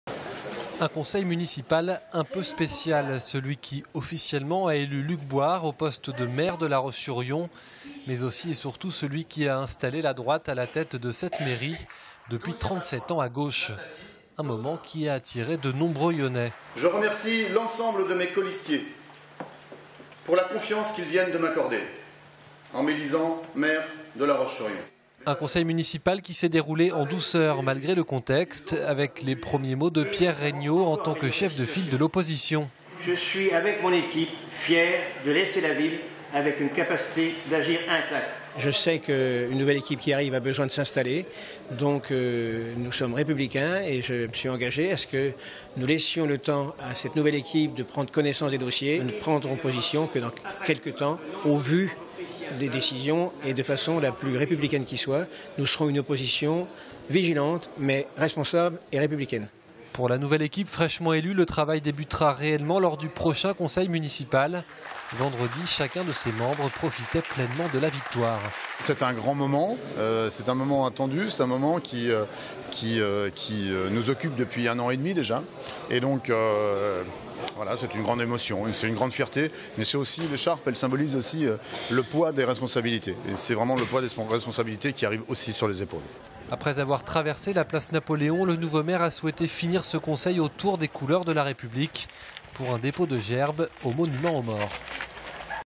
La Roche-sur-Yon bascule à droite: Luc Bouard, nouveau maire
Sites webs/blogs, copiez cette vidéo avec le bouton "Intégrer" !
 Après 35 ans, La Roche-sur-Yon bascule à droite ! Le maire UMP, Luc Bouard, a pris ses fonctions vendredi.